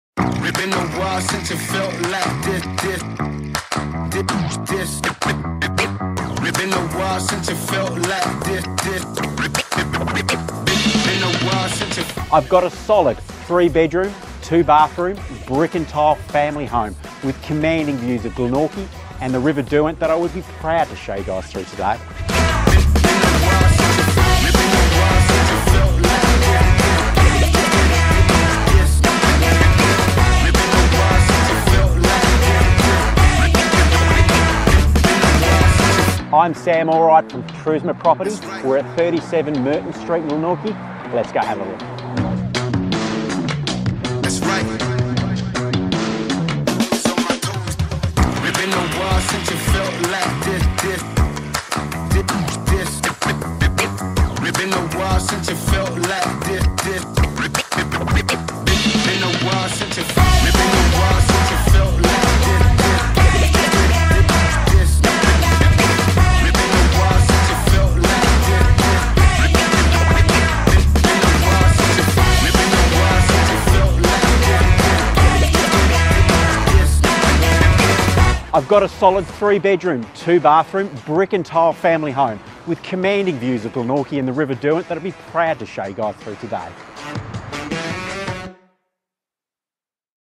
37 Merton Street in Glenorchy
37 MERTON STREET IN GLENORCHY
This remarkable 3-bedroom 2-bathroom brick & tile family home has a commanding street appeal with stunning views of Tolosa Park, Glenorchy and the Derwent River itself. The concrete driveway winds up through immaculately terraced front gardens to present the homes main entrance, a two-car garage alongside and parking for at least three vehicles outside.

Inside the entry foyer we have a magnificent parents retreat and stairs that lead up to the second level. The recently completed renovations include the master bedroom with a back-lit headboard, back-lit walk though robe and ensuite that really must be seen in person. Upstairs we have two separate living areas with full length windows to let the all-day sun in or just to enjoy those stunning views. Two large bedrooms, both with built-ins and an even bigger main bathroom complete a family home to be proud of.

The kitchen is updated & immaculate, the home has the romance and warmth of a gas fire, but the convenience and year-round comfort of an extensive multi-room ducted air-conditioning. This leads out onto the pergola that is perfect for warm summer evenings. Outside we have multiple decks to make the most of those views and the gardens themselves are perfect for any avid gardener with a multitude of mature fruit trees, garden sheds, paved or concrete paths and raised garden beds.

Perfectly positioned and so close to schools, public transport, the revitalised Glenorchy shopping precinct and only 5 minutes away are major attractions like MONA. So please reach out me now as I would love to take you through and make this home yours.

• Sunning parent's retreat.
• Extensive multi-room ducted air-conditioning.
• Commanding street appeal with stunning views.
• Immaculate terraced gardens & raised garden beds.
• Large two car garage and additional storage.